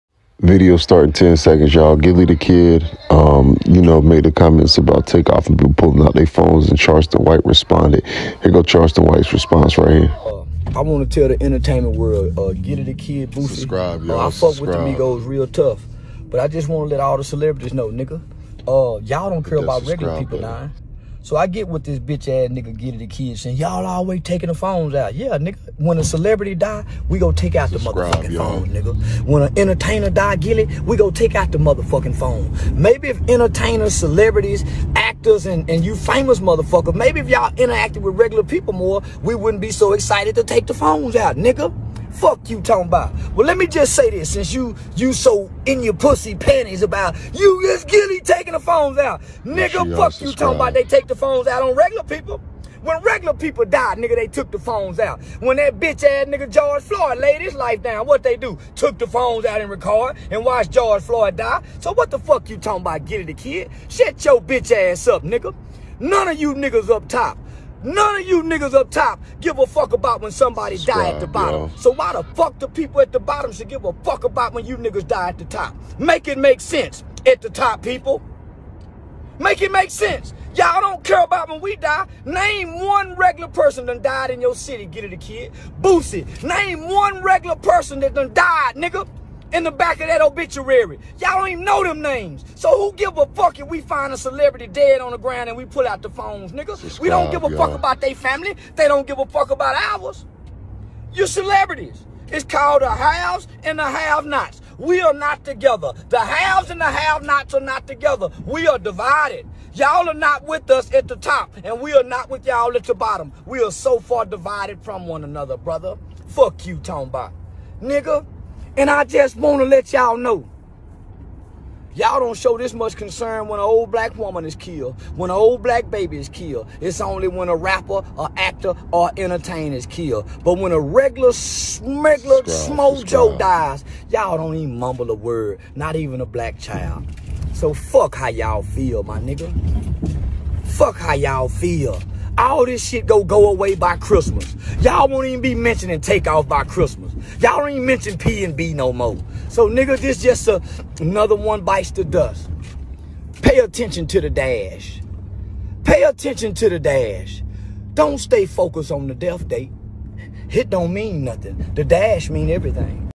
Charleston white goes in on gillie the kid, speak on takeoff
BIGCEOSOLO speaks on why Charleston white goes in on gillie the kid